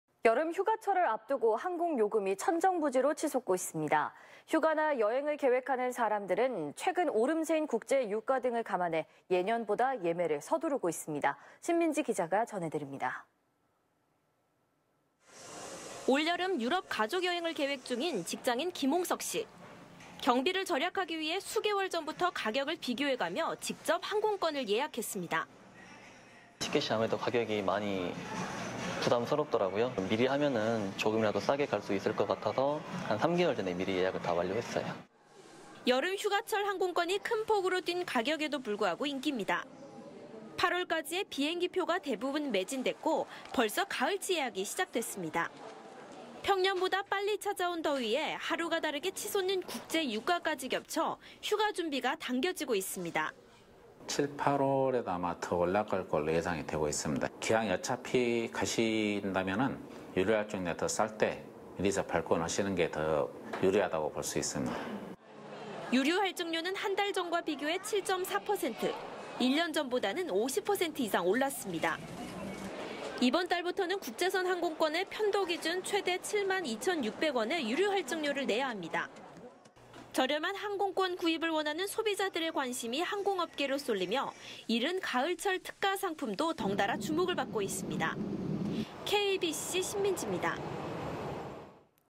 휴가철 앞두고 항공 가격 '더 오를까'..예매↑
여름 휴가철을 앞두고 항공 요금이 천정부지로 치솟고 있습니다.

휴가나 여행을 계획하는 사람들은
최근 오름세인 국제유가 등을 감안해
예년보다 예매를 서두르고 있습니다.

신민지 기자가 전해드립니다.